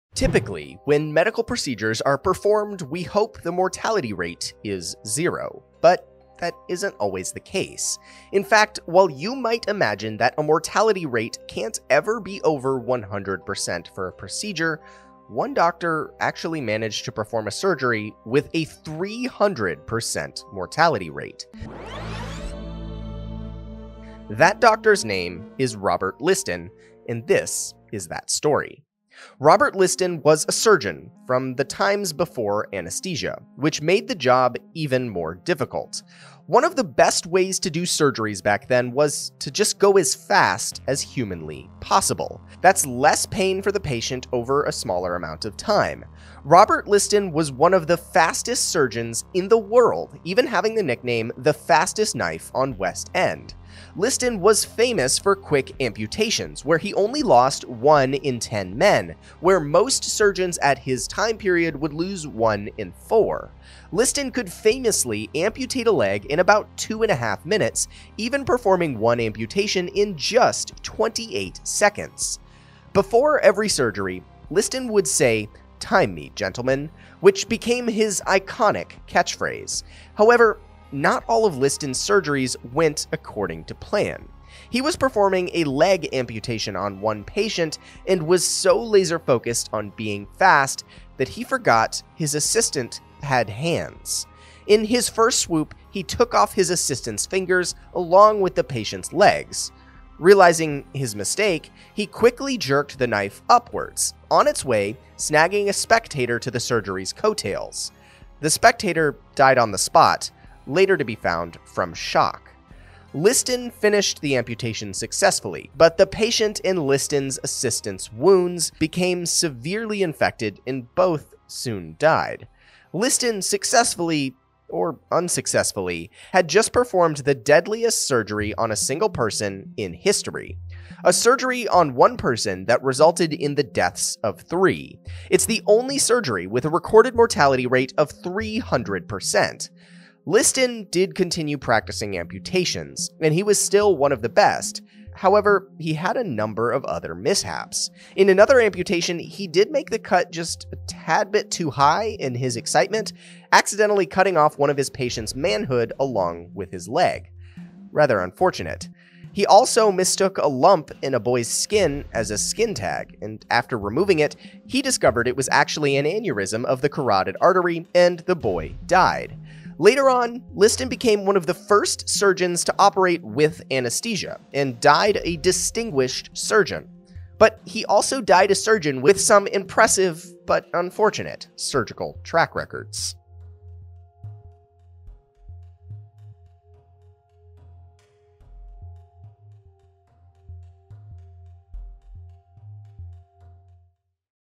The Surgery with a 300% Mortality Rate
Typically when medical procedures are performed we hope the mortality rate is 0, but that isn’t always the case. In fact, while you might imagine that a mortality rate can’t be over 100% for a procedure, one doctor actually managed to perform a surgery with a 300% mortality rate. That doctor’s name is robert Liston and this is that story.

Strange Stuff